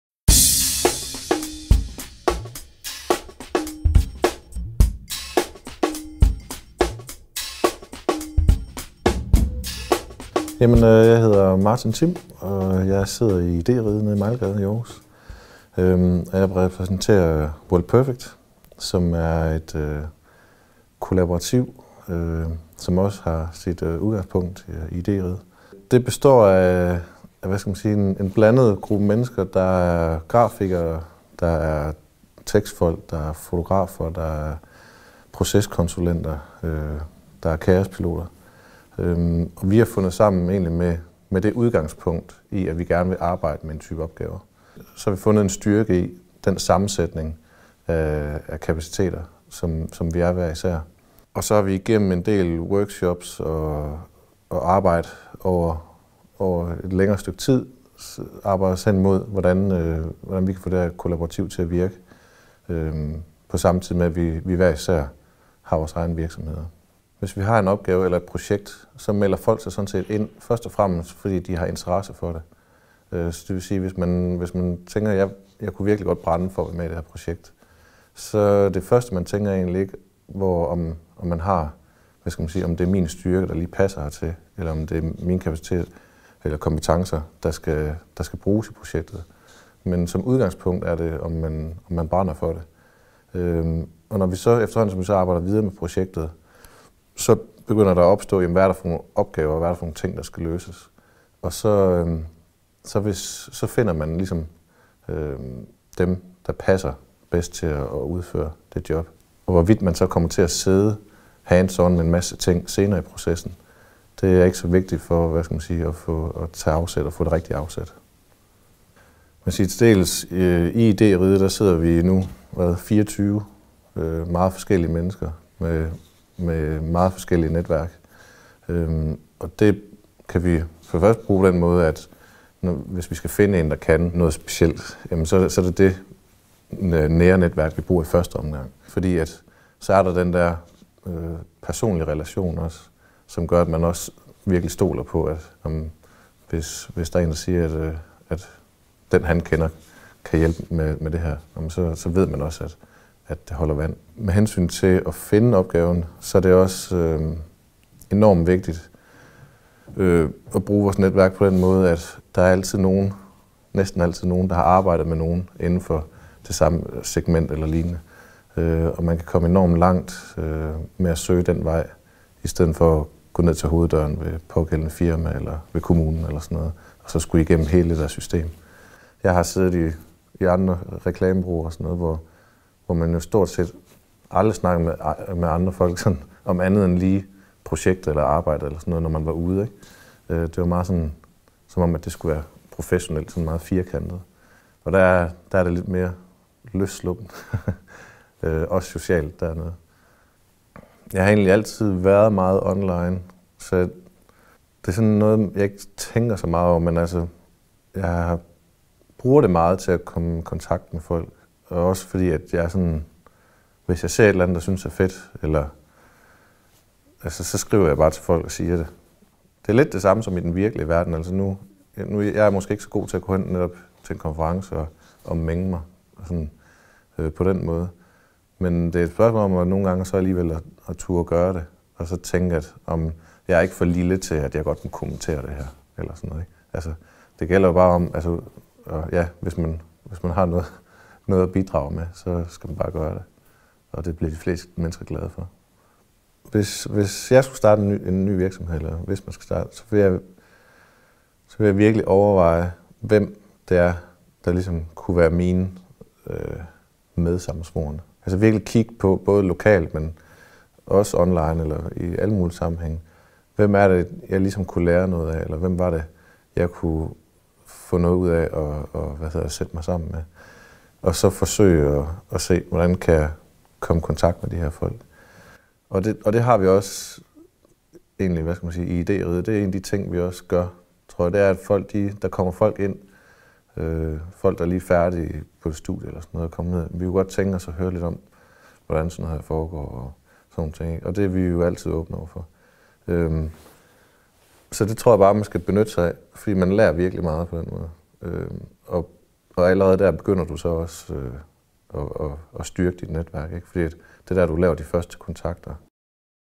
Martin Thim fortæller om netværk og networking
Martin Thim, der bl.a. er en del af netværksvirksomheden World Perfect fortæller, hvordan de bruger netværk til fordeling og udvikling af nye projekter og opgaver. Læs mere om World Perfect på